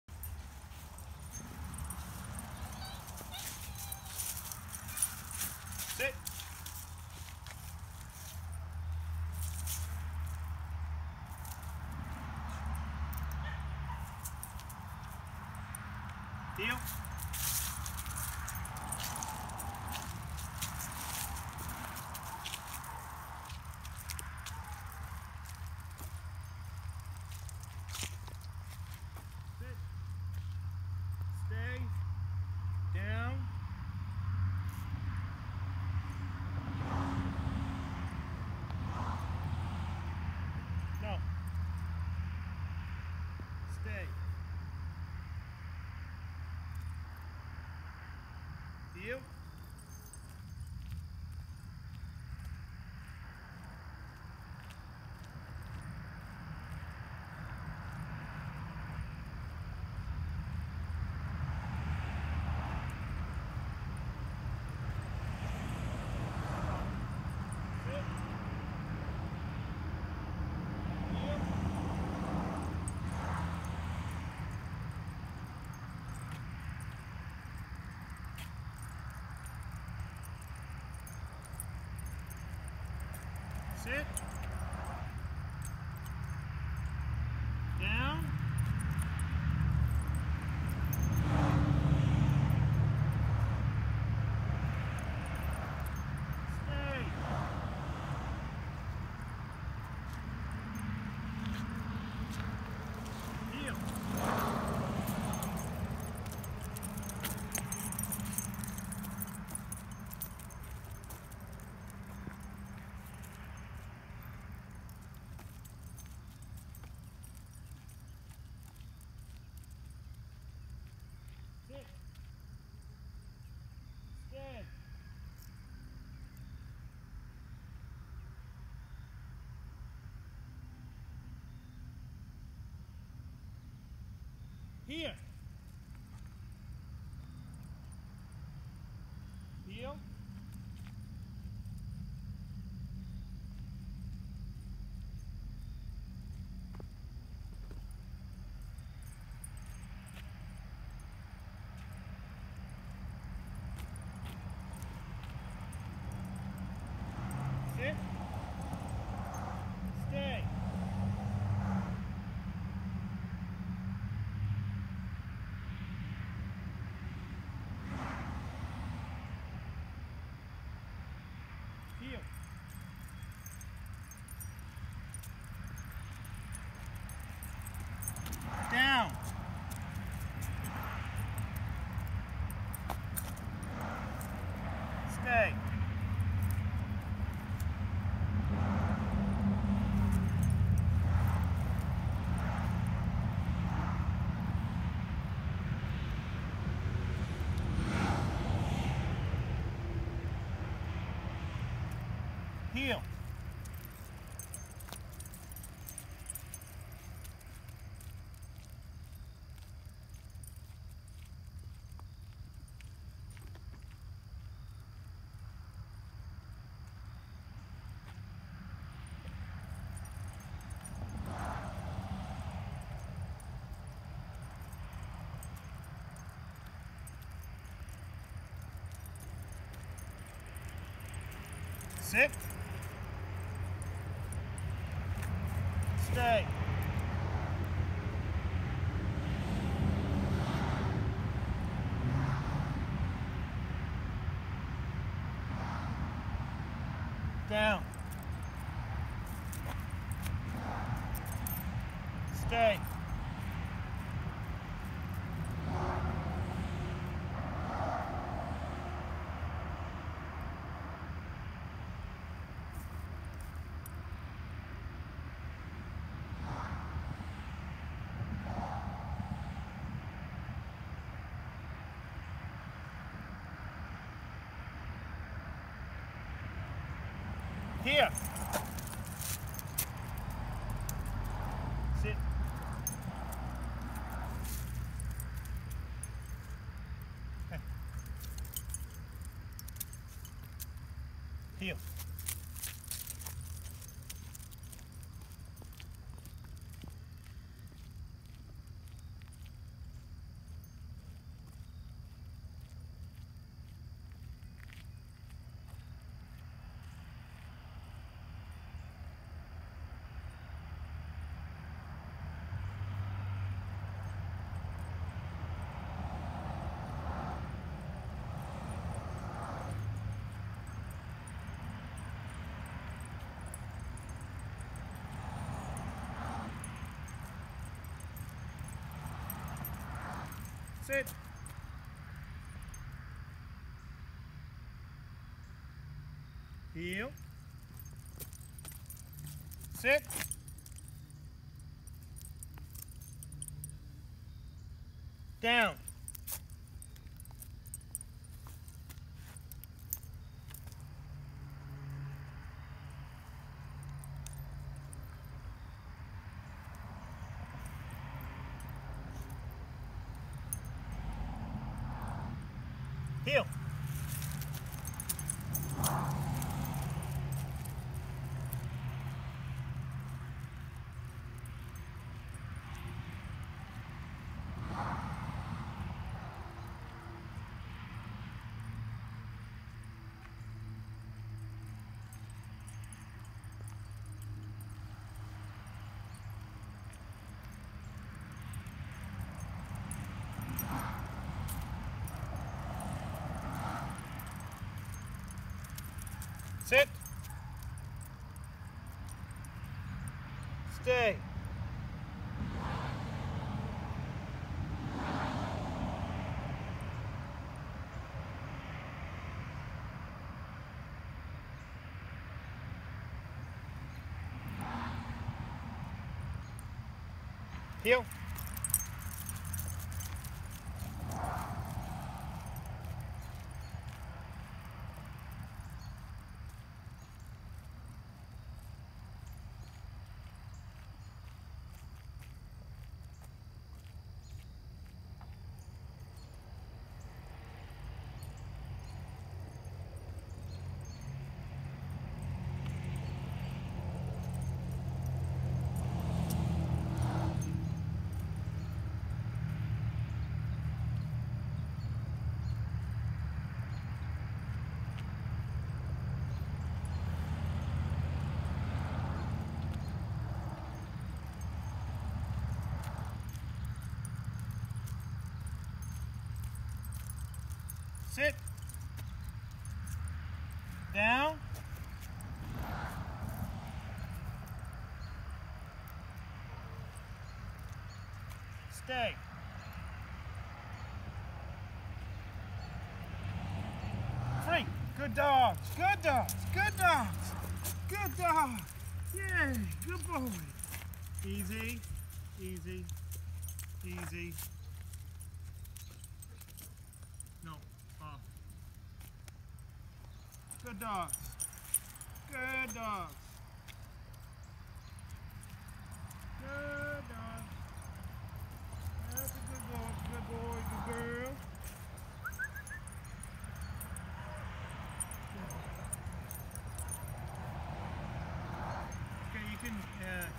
Video 1
Boot camp video 4 weeks in house, not the same dog's they had so many issues ! fear, dog aggression ,anxiety, dominance